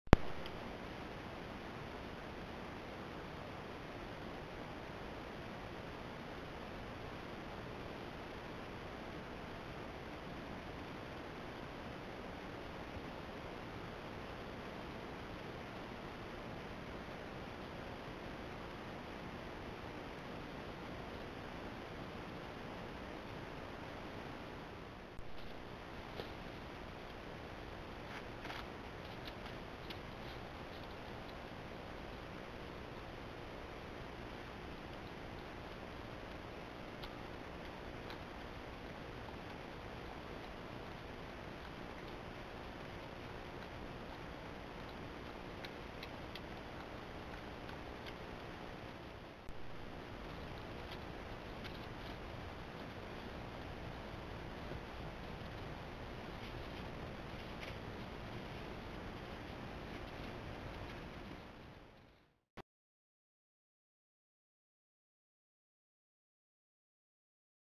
Badger's rare winter appearance, legs are too short in the snow :-)
The first clip shows the animal before the snow fell. It seems to always be busy checking things out. Then the snow fell and badger's short legs don't work too well in the deep snow. The critter clearly struggles at the end of the video. It was, however, mighty thirsty and spent over a minute drinking from the little melted snow. These videos are from our trail camera on the Colorado Eastern Plains.